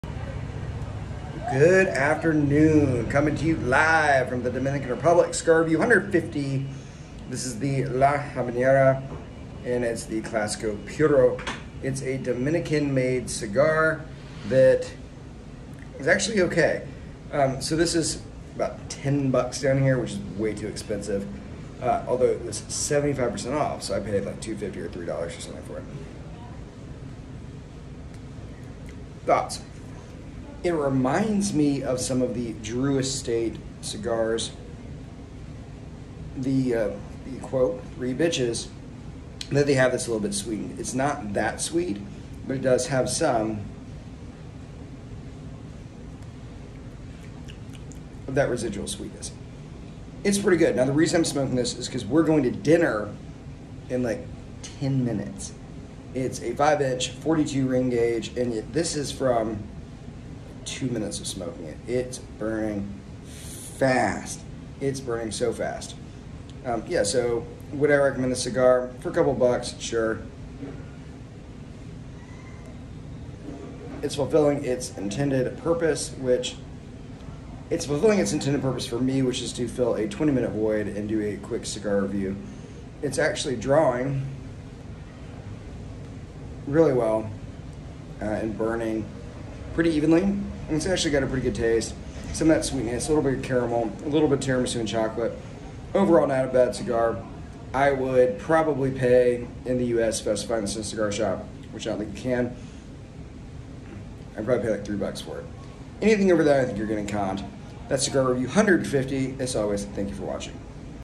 Cigar Review 150: La Habanera Classico Puro
Decent but too expensive.  Conclusion: Don’t buy